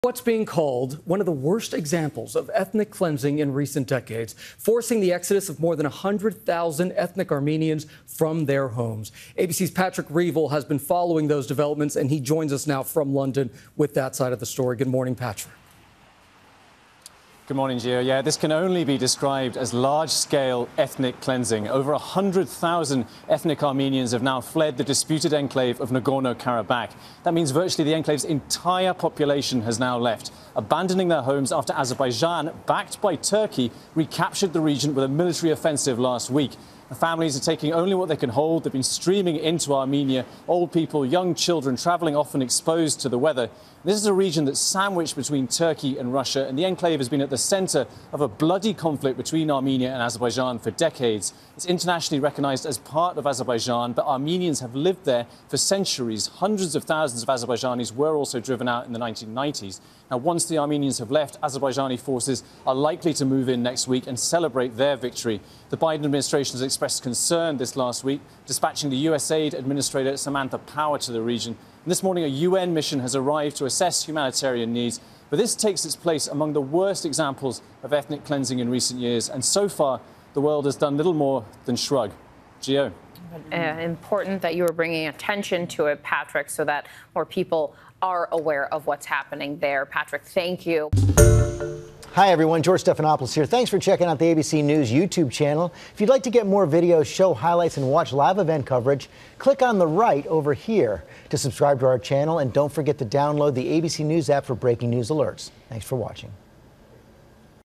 More than 100,000 Armenians flee in mass exodus | GMA
Over 100,000 ethnic Armenians have now fled the disputed enclave of Nagorno-Karabakh after Azerbaijan, backed by Turkey, recaptured the region with a military action last week.